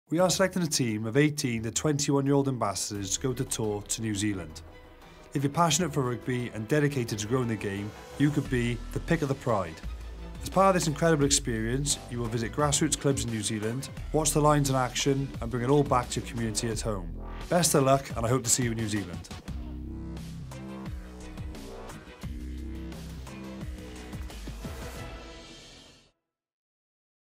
Sam’s call to join the Pick of the Pride | The British & Irish Lions
British & Irish Lions Captain Sam Warburton has issued a rallying roar for 18-21 year-olds across the four Unions to join the ‘Pick of the Pride’ and go on Tour in New Zealand!

To find out how you can enter the competition, go